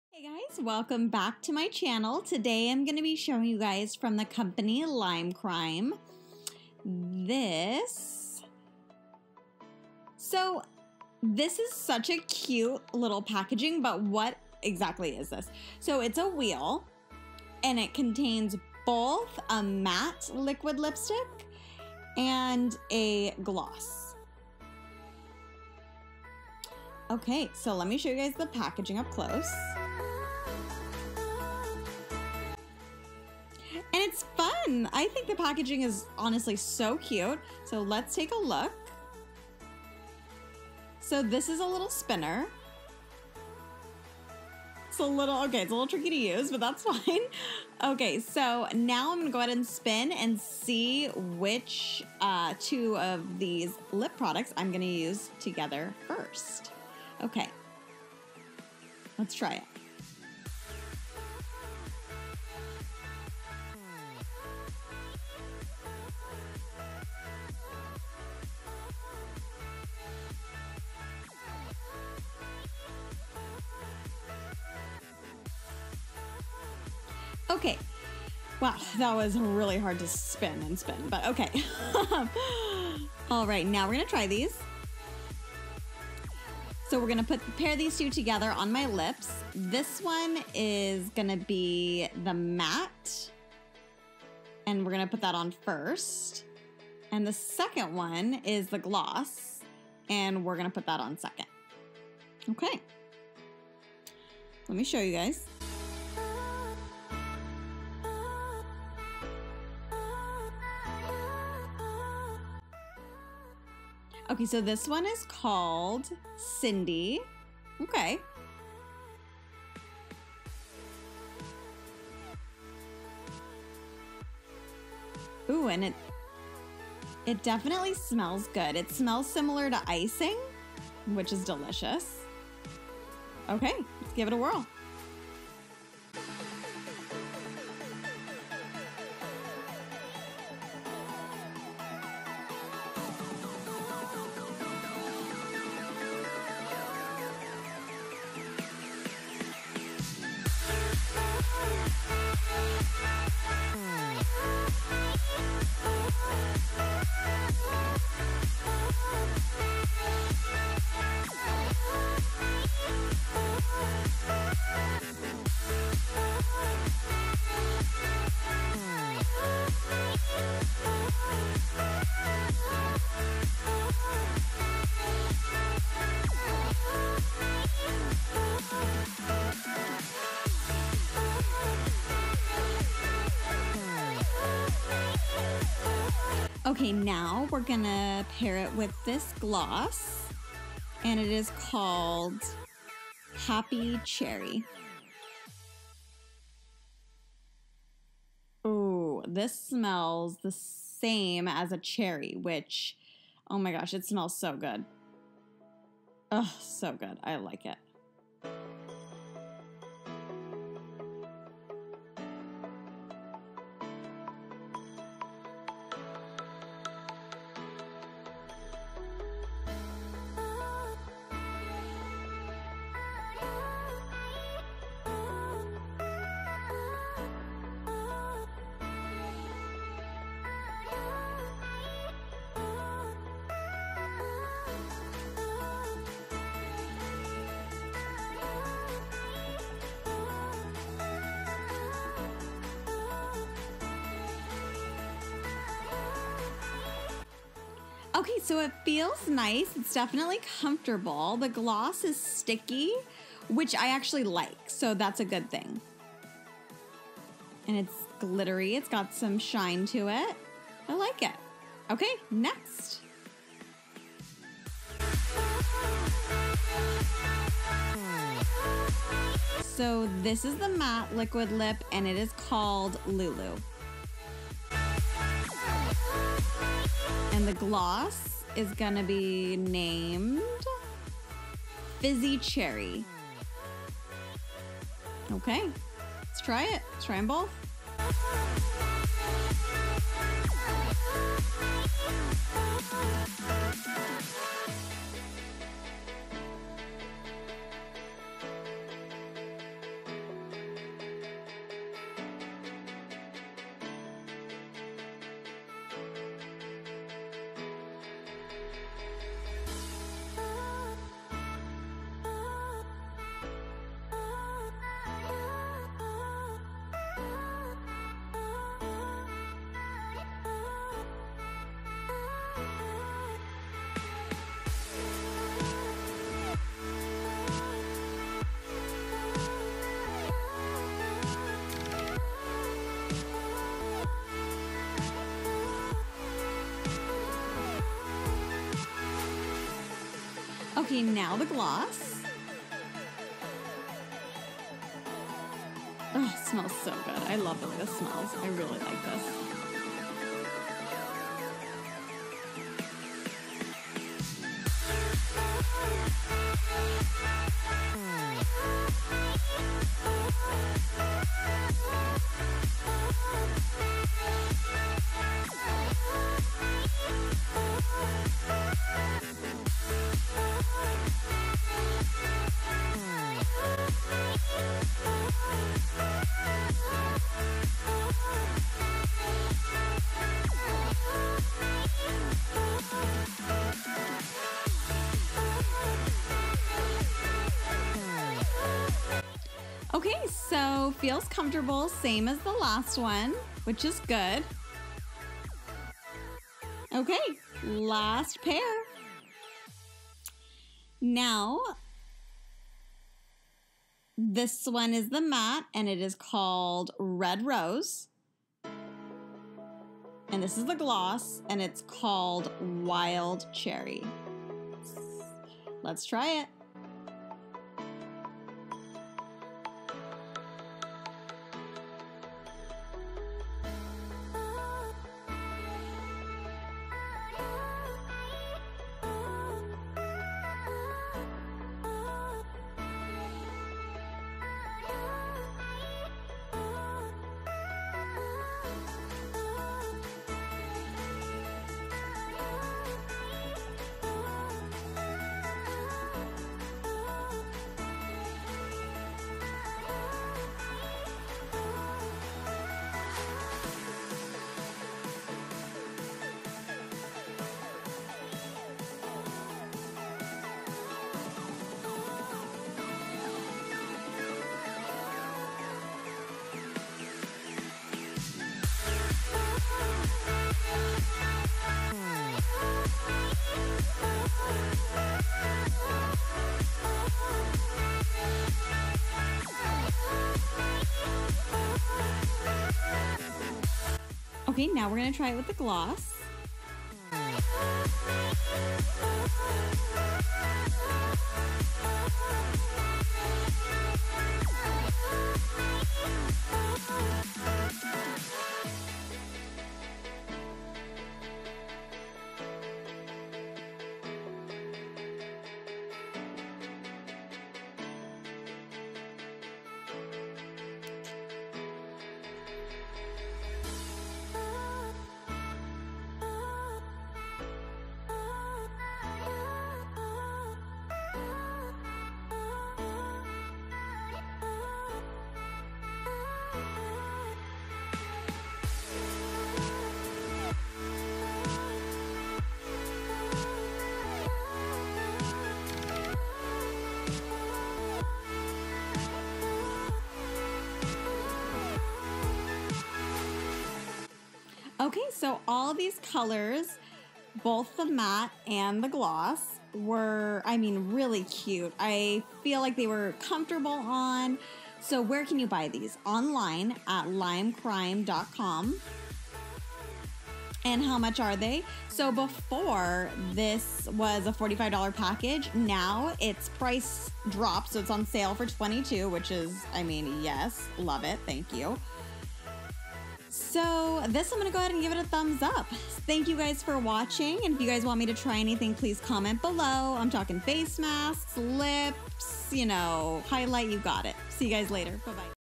ASL Review: Lime Crime Spin the Dial Lip Set
American Sign Language Review of Lime Crime Spin the Dial Lip Set. Swatches of matte liquid lipsticks and lip glosses!